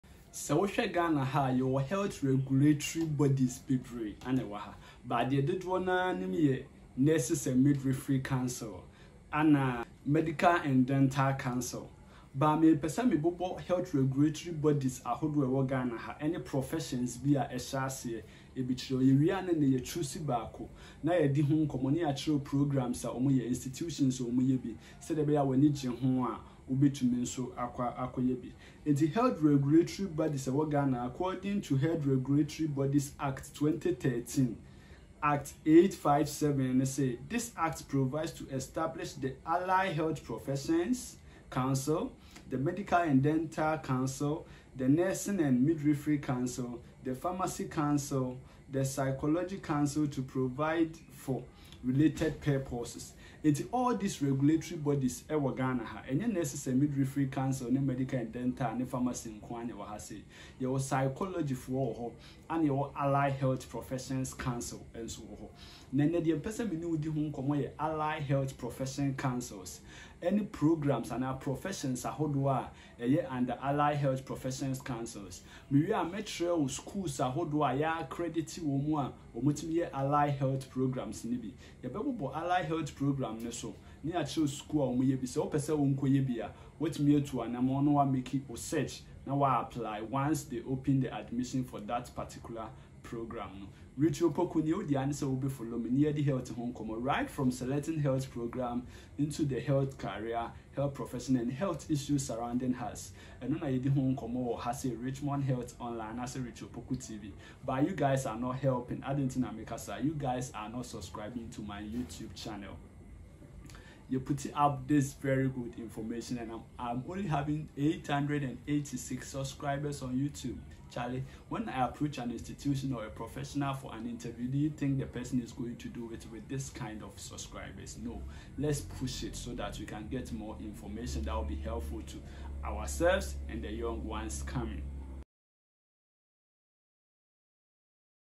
Allied Health Accredited Institutions and Programs they offer
Allied Health Professionals are encouraged to ensure that the programmes they intend to attend are duly accredited by the Council.